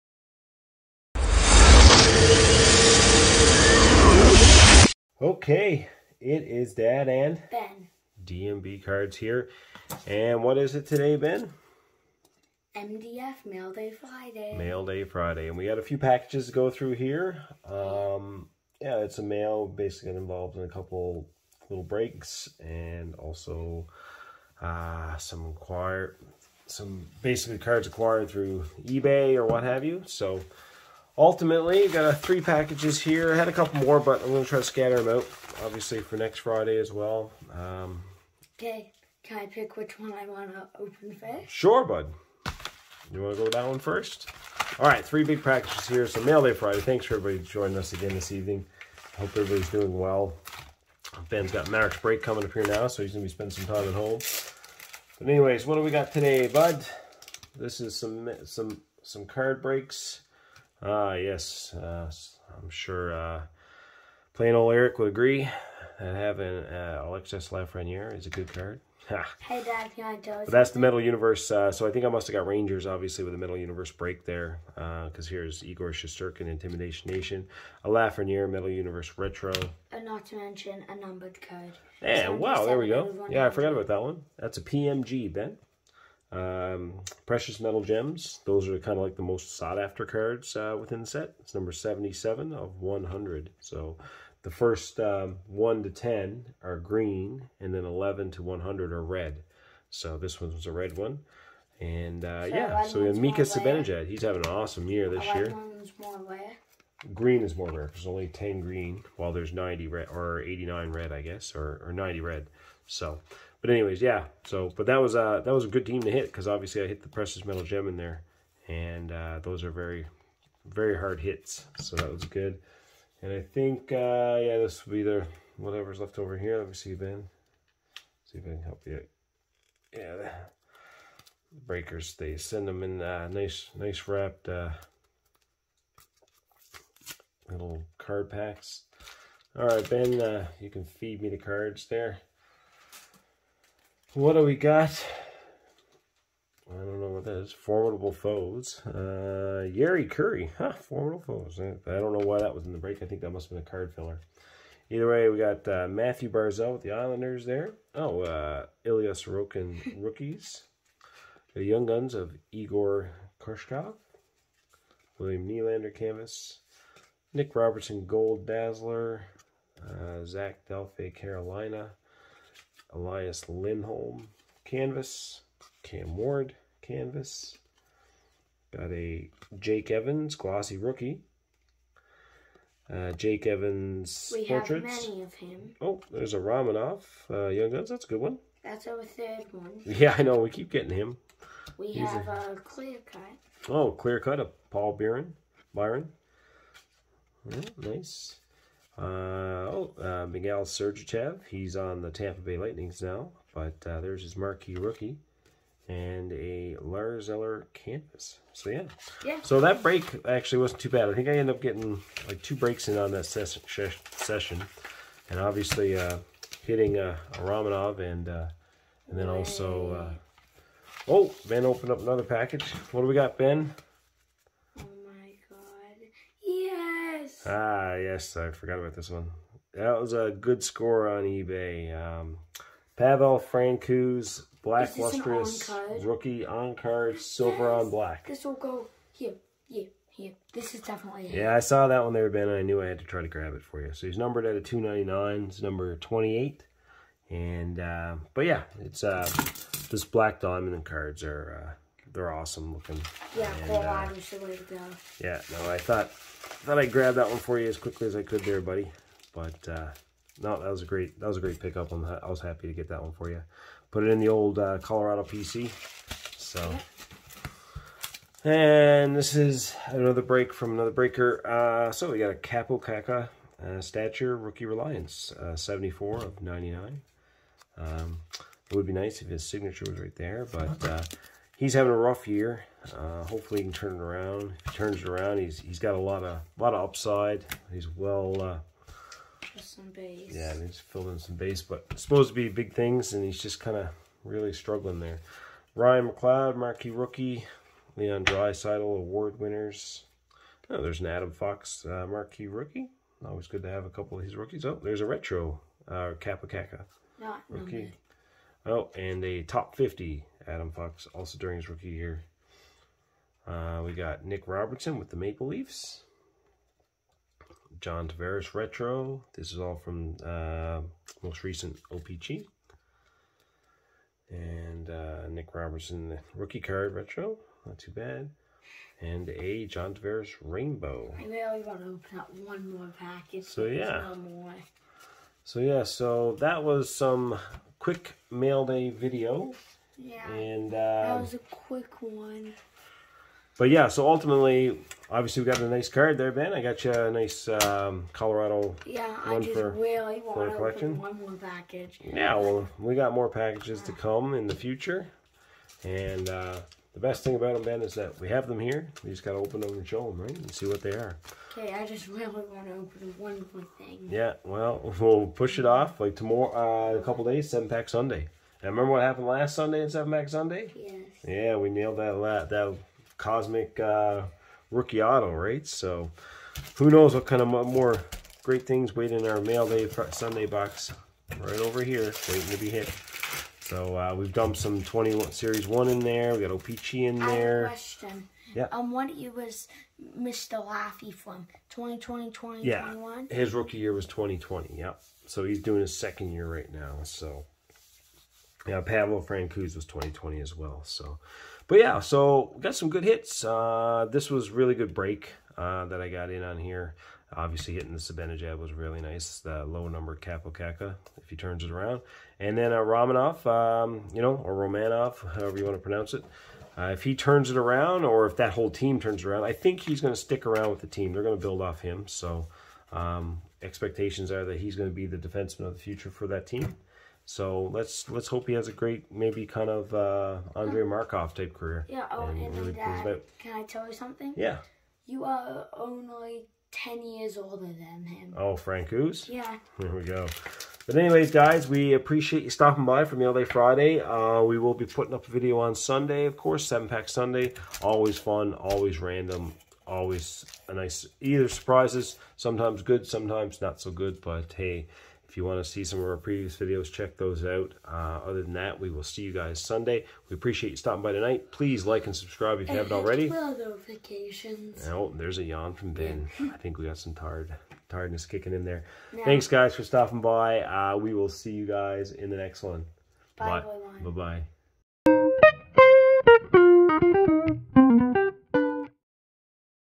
Mail Day Friday - Young Guns, Rookie Auto, PMG and more. :)
Ben and I tear into some mail. Real nice pickups. :) Ben was thrilled with a card pickup. We hope you enjoy.